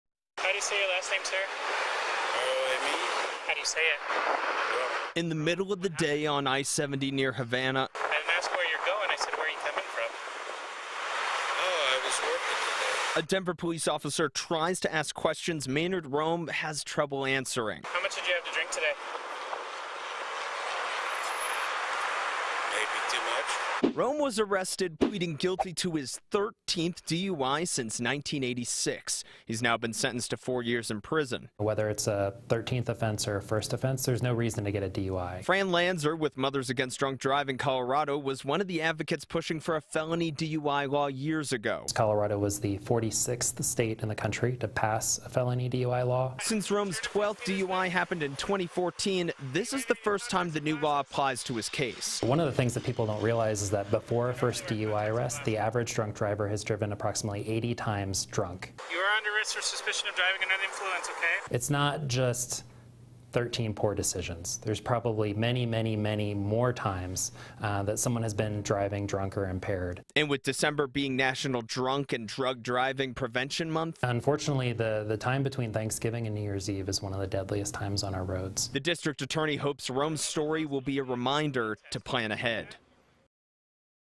Denver man gets 4-year prison sentence after 13th DUI
A man was sentenced to four years in prison after he pleaded guilty to his 13th DUI. Alex Rose reports